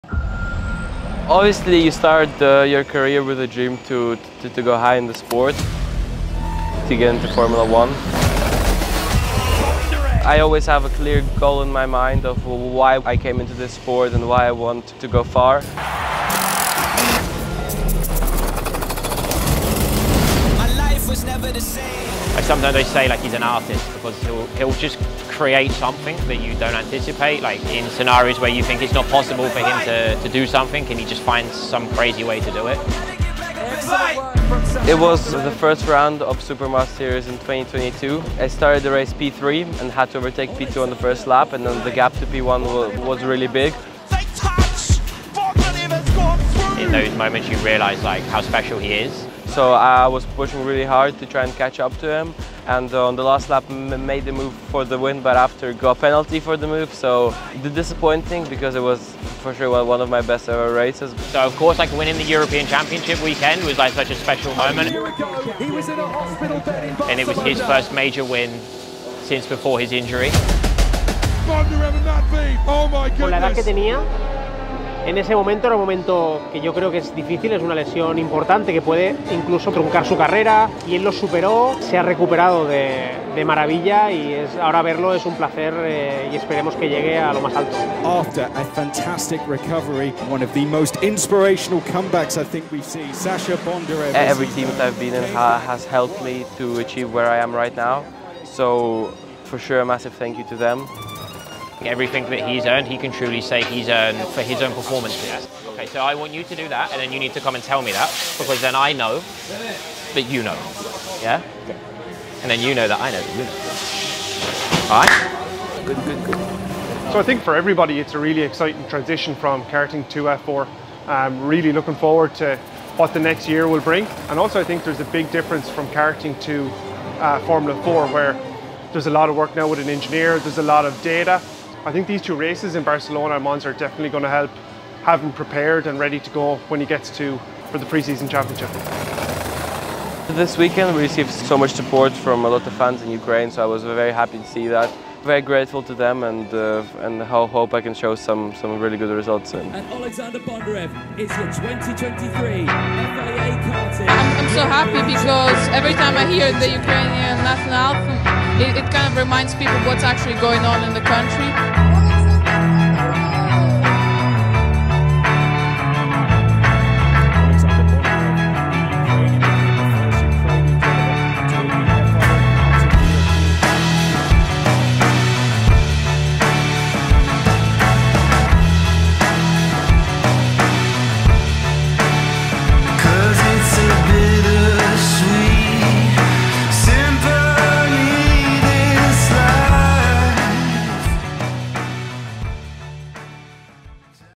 Oleksandr Bondarev: “Wrapping up my 10 years in karting…” | Interview
Oleksandr Bondarev — Ukrainian pilot of Italian F4

Wrapping up my 10 years in karting. Feeling very delighted and super grateful to everyone who was involved in this journey. Onto a new adventure in cars!
▪️ Director/Producer: Dasha Golubkova
▪️ Edit: Alex Melnikov